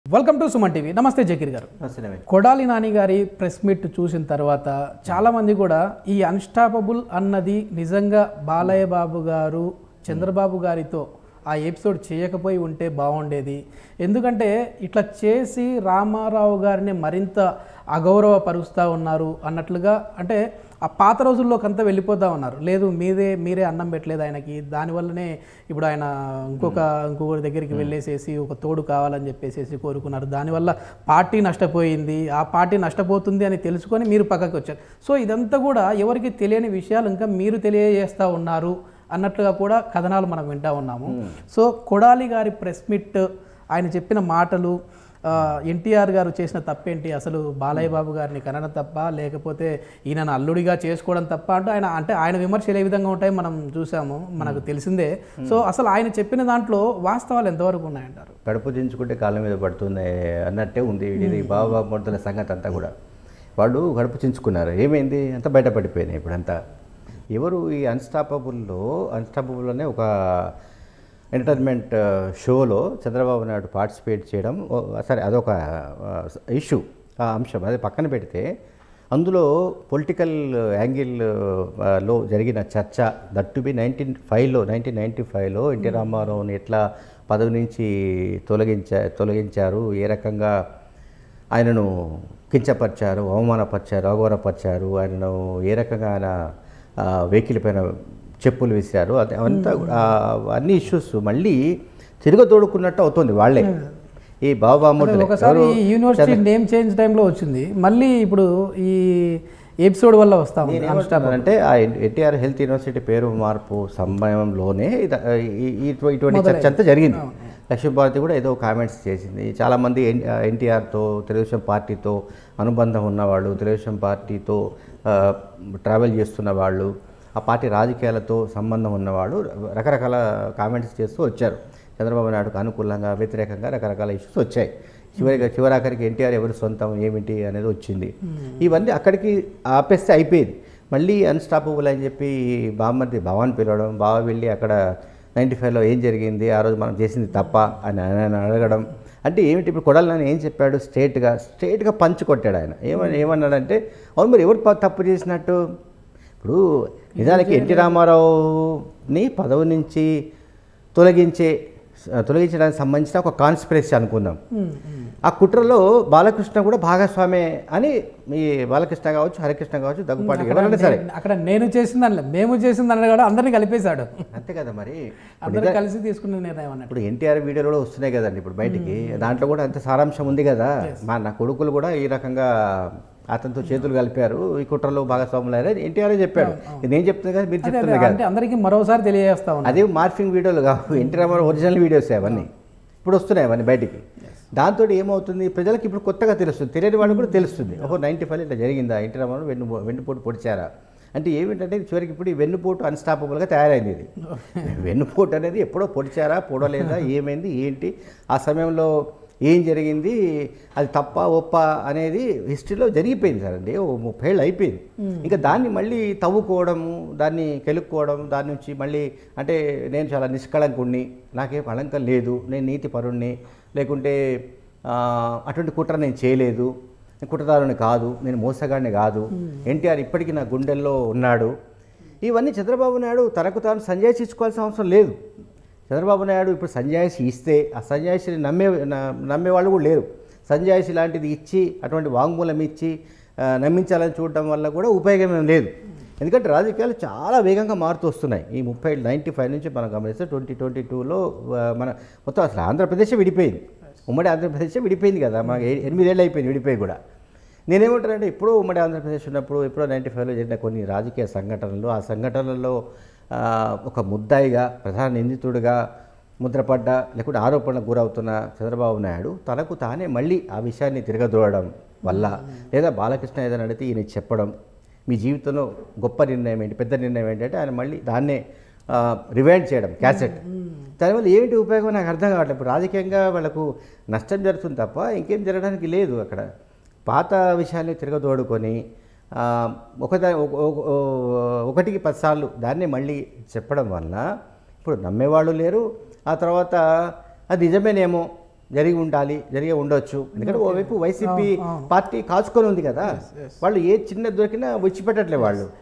Analyst Zakeer About Kodali Nani Comments On Unstoppable Episode | Chandrabaub | Balakrishna
Watch► Analyst Zakeer About Kodali Nani Comments On Unstoppable Episode | Chandrabaub | Balakrishna @sumantvtelugulive